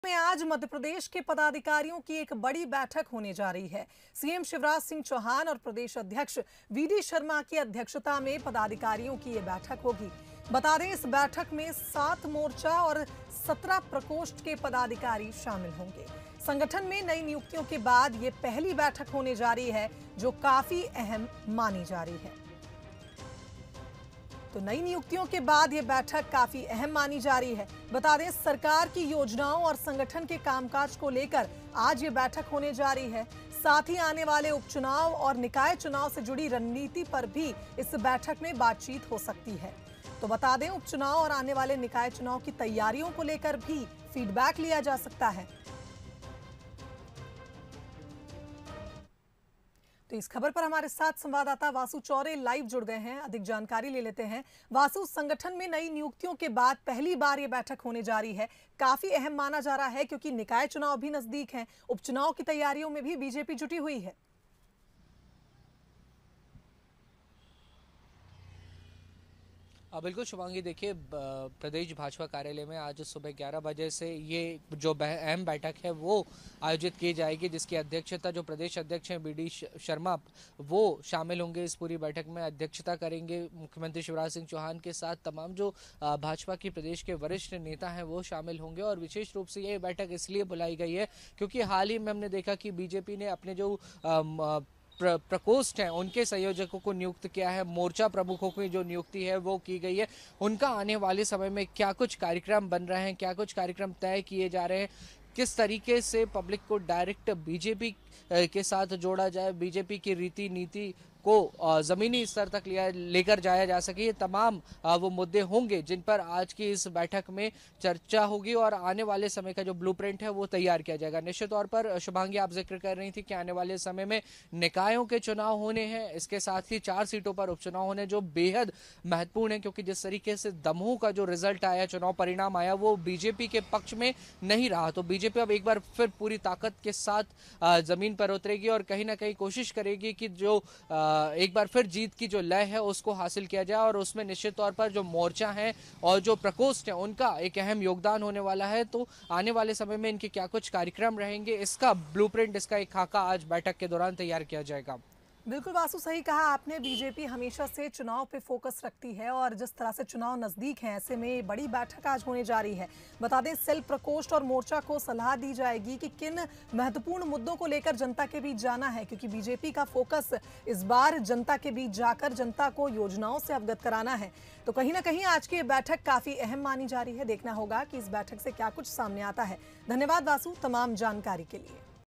नई नियुक्तियों के बाद अहम बैठक, आज होगी बीजेपी की बड़ी बैठक
भोपाल में आज मध्य प्रदेश के पदाधिकारियों की बड़ी बैठक होगे जा रही है.सीएम शिवराज सिंह चौहान और प्रदेश अध्यक्ष वीडी शर्मा की अध्यक्षता में पदाधिकारियों की बैठक होगी.

Incoming Search Terms:

Bansal News, Bansal News Madhya Pradesh, Bansal News Chhattsigarh, all hindi news, नई नियुक्तियों के बाद अहम बैठक, आज होगी बीजेपी की बड़ी बैठक